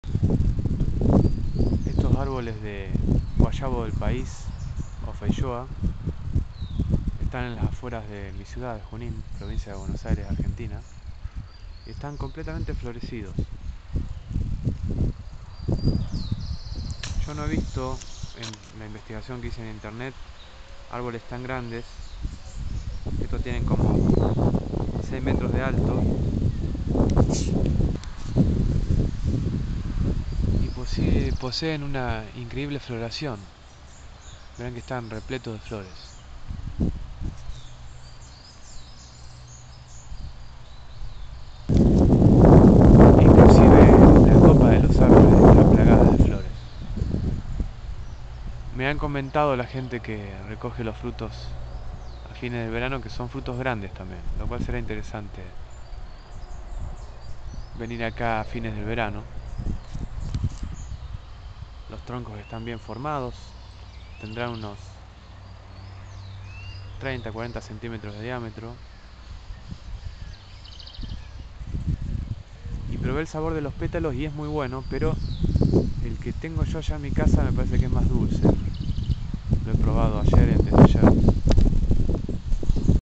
Guayabo del país, feijoa (acca sellowiana, pineaple guava)
Un impresionante guayabo del país, frutal sudamericano muy nutritivo, rico en vitamina C, potasio y antioxidantes, de exquisito sabor mezcla banana-manzana-frutilla